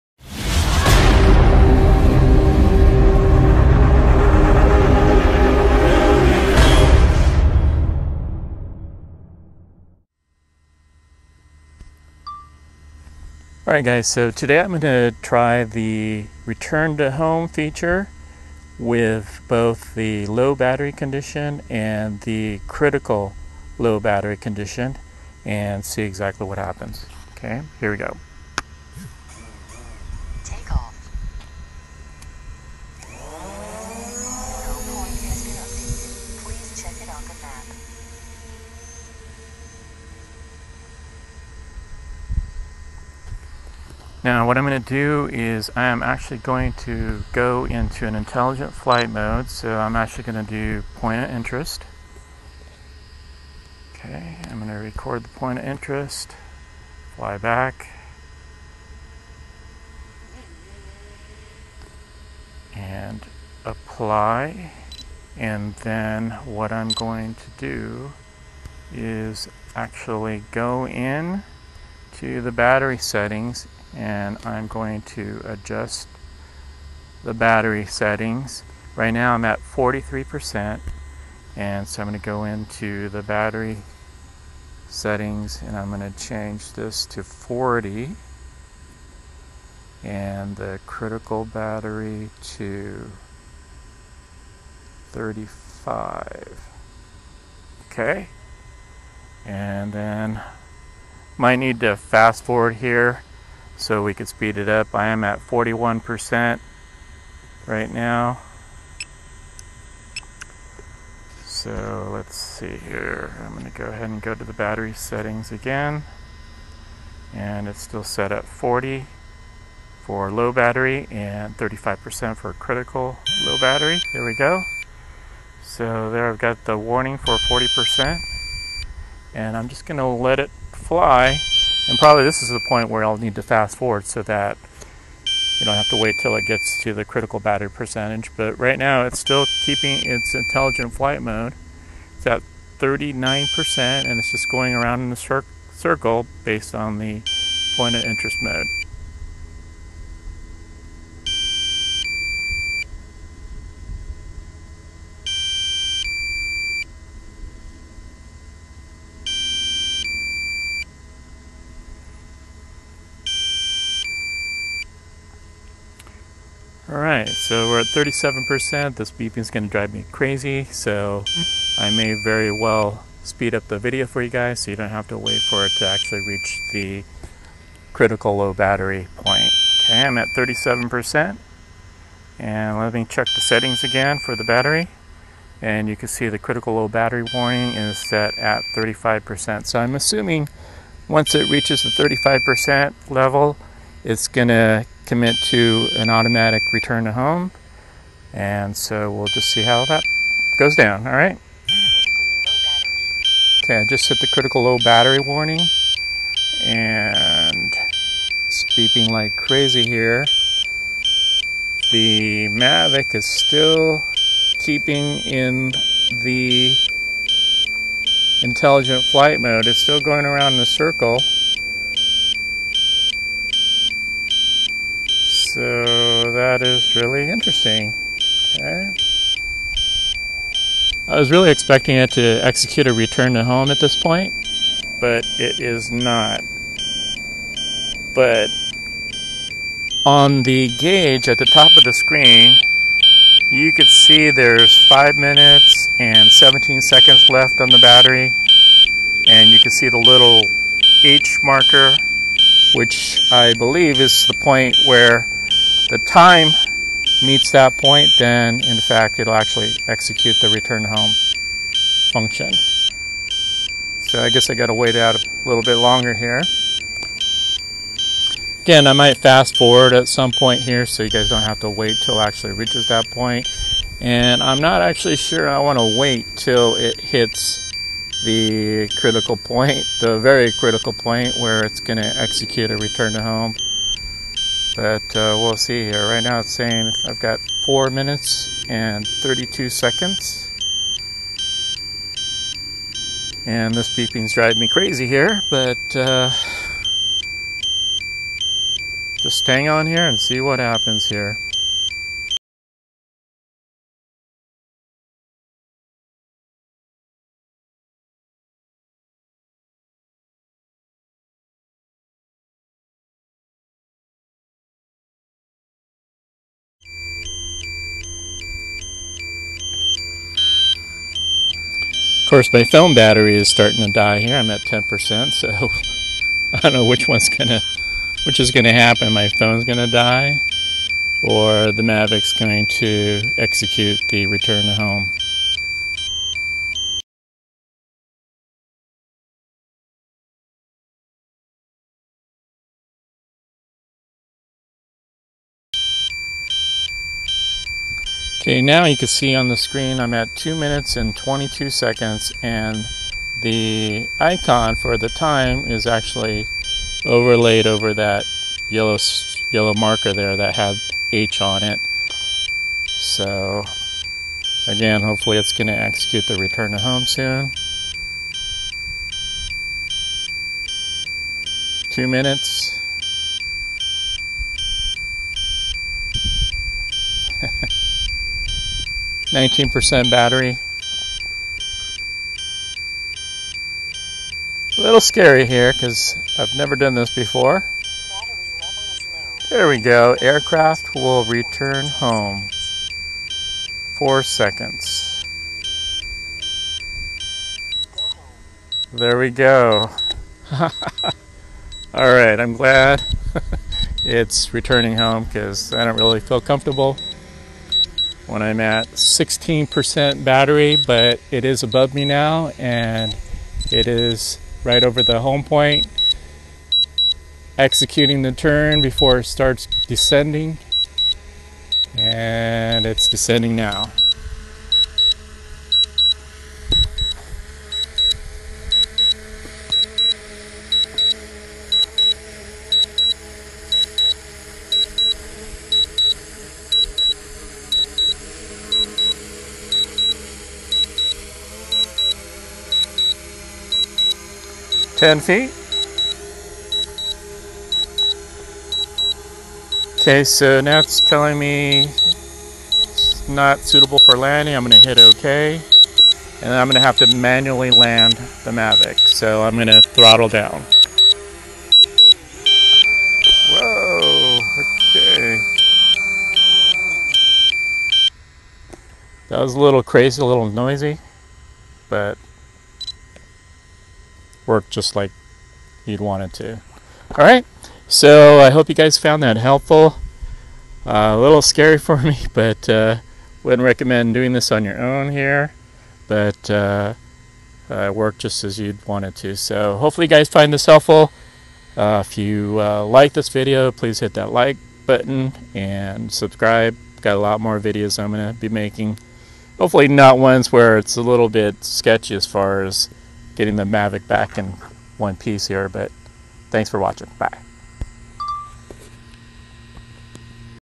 Flying the DJI Mavic Pro under ALL Low Battery Conditions
This is a test of the DJI Mavic Pro in-flight, and under all low battery conditions.

There are 3 low battery conditions:  Low Battery, Critical Battery and Low Battery Return-to-Home.  These conditions are met while in flight, and while operating in the Point Of Interest POI flight mode.

NOTE:  I would not recommend any pilots in general to recreate this test, as flying under very low battery may have different and undesirable results depending on the actual age/condition of the battery.

DJI GO 4 app version 4.0.2.

I hope you found this video helpful, and provides you with more confidence knowing what happens during low/critical battery conditions.